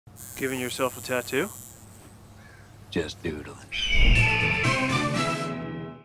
Mike’s HARDER Lemonade - Doodling :06
The HARDER Farmer wasn’t programmed to feel pain. His only purpose is to unleash can after can of Mike’s HARDER Lemonade on the world’s taste buds.